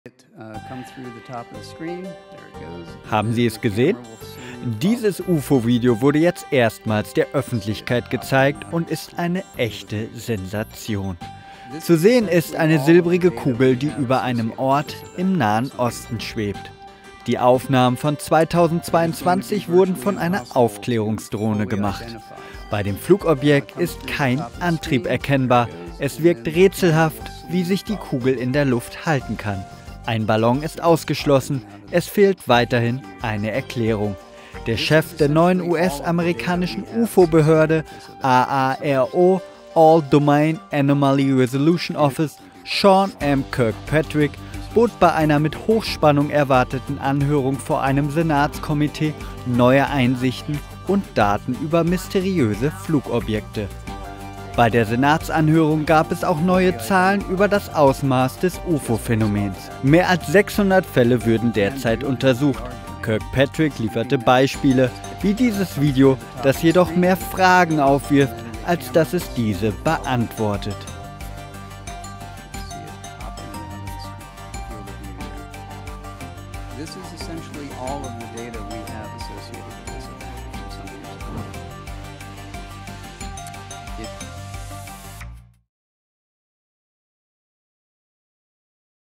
Rätsel um UFO-Kugel: Was zeigt dieses Video?
Dieses UFO-Video wurde jetzt erstmals der Öffentlichkeit gezeigt und ist eine echte Sensatio: Zu sehen ist eine silbrige Kugel, die über einem Ort im Nahen Osten schwebt.

Die Aufnahmen von 2022 wurden von einer Aufklärungsdrohne gemacht. Bei dem Flugobjekt ist kein Antrieb erkennbar. Es wirkt rätselhaft, wie sich die Kugel in der Luft halten kann. Ein Ballon ist ausgeschlossen. Es fehlt weiterhin eine Erklärung!

Der Chef der neuen US-amerikanischen „Ufo-Behörde“ AARO (All-Domain Anomaly Resolution Office), Sean M. Kirkpatrick, bot bei einer mit Hochspannung erwarteten Anhörung vor einem Senats-Komitee neue Einsichten und Daten über mysteriöse Flugobjekte.

Bei dem Senats-Hearing gab es auch neue Zahlen über das Ausmaß des Ufo-Phänomens: Mehr als 600 Fälle würden derzeit untersucht. Kirkpatrick lieferte Beispiele. Wie dieses Video, dass jedoch mehr Fragen aufwirft als dass es diese beantwortet.